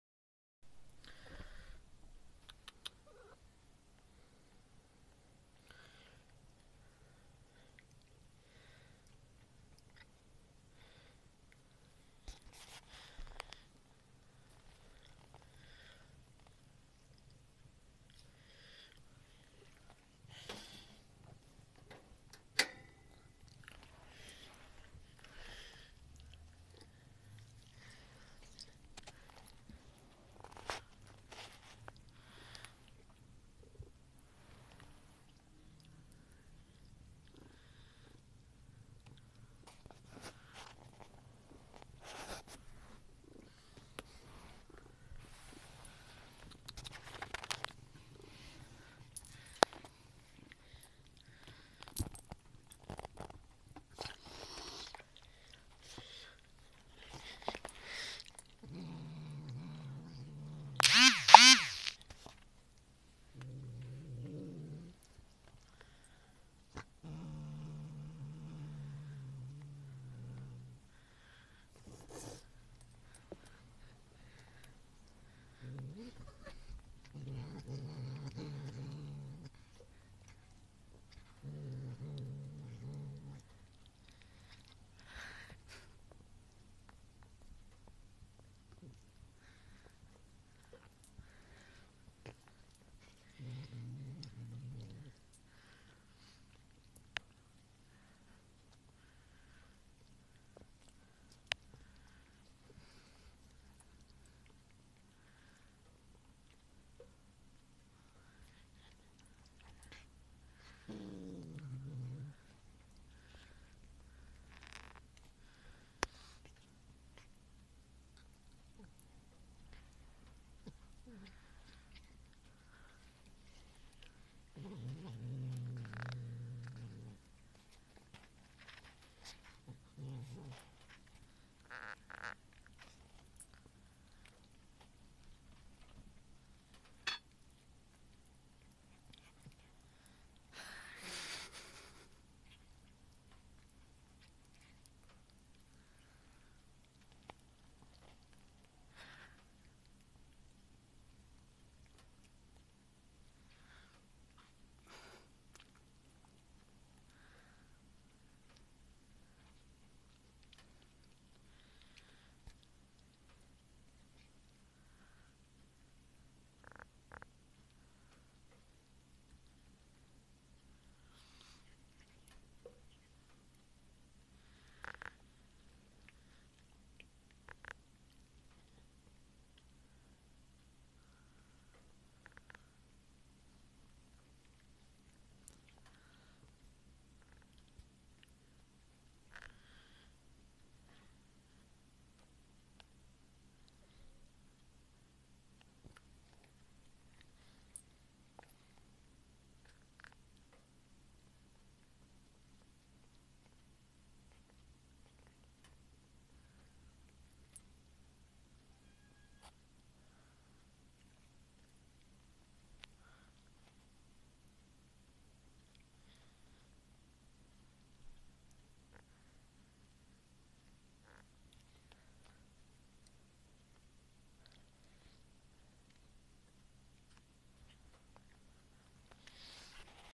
The growling cat
Ronan tries to eat a piece of cheese and growls at Muff even after she's gone. In doing so, he makes funny noises while eating.